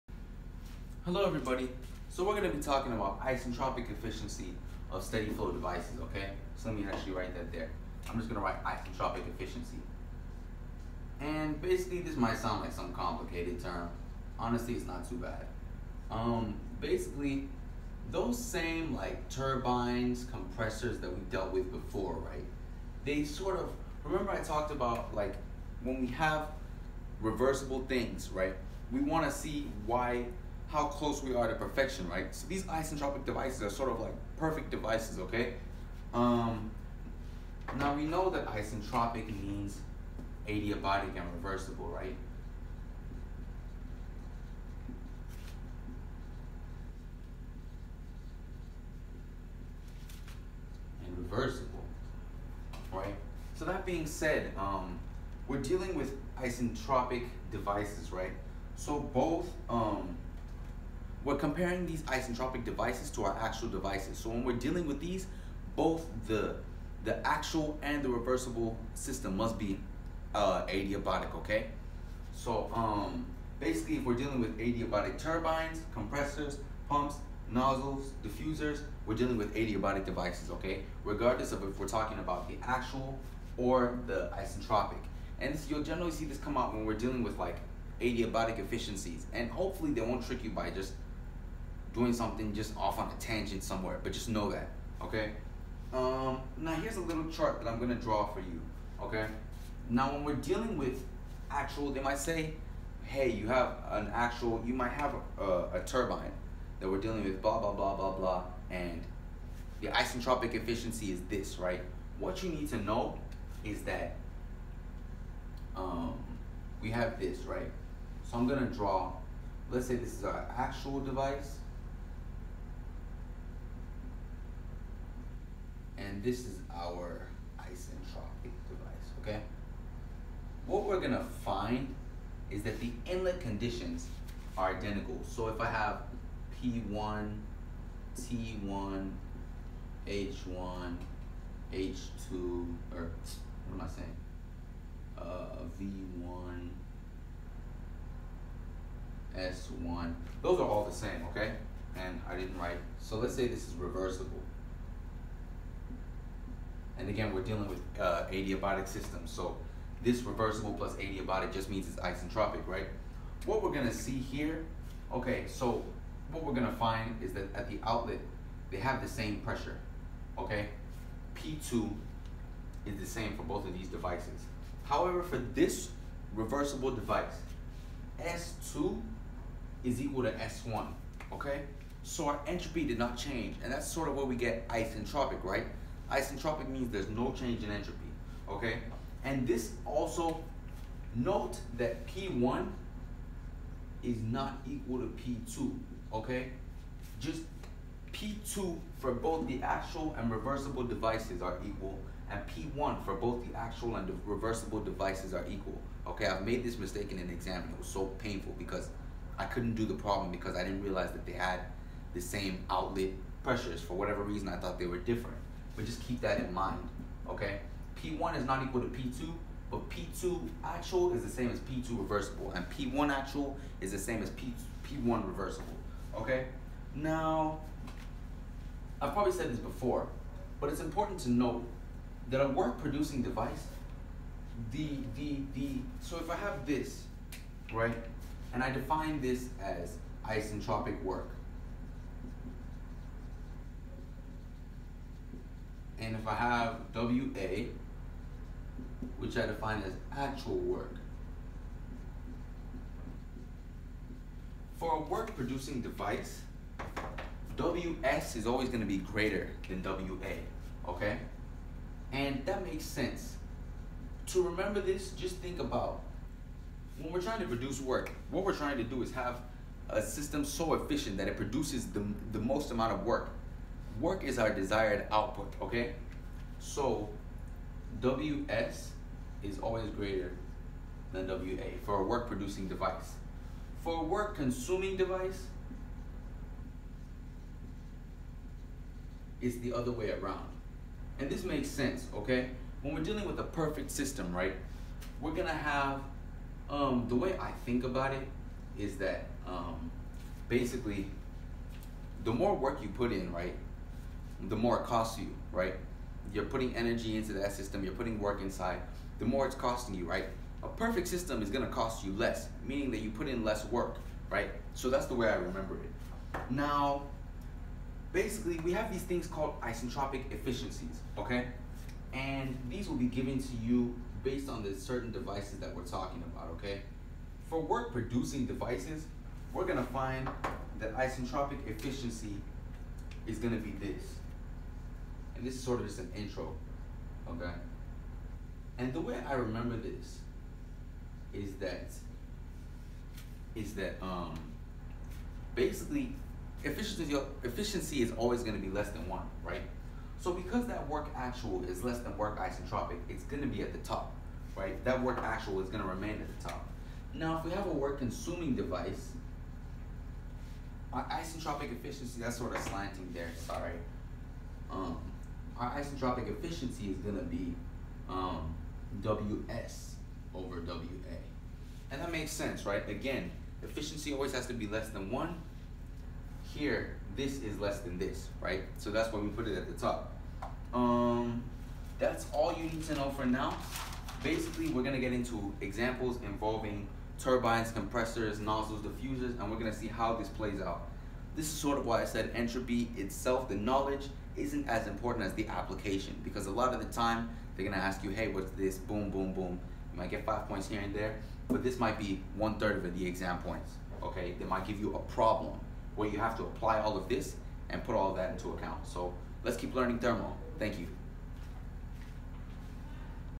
Lesson 17: Isentropic Efficiencies
An introduction into isentropic efficiencies. This should give you a brief overview before looking at isentropic efficiencies for specific devices such as turbines and compressors.